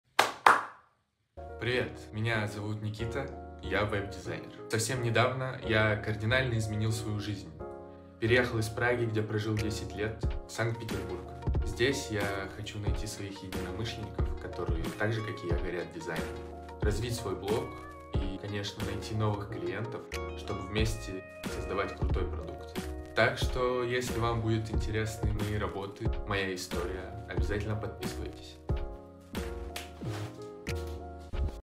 переехал из Европы в Питер short
Меня зовут Никита
И я веб-дизайнер
Совсем недавно, я переехал из Европы в Питер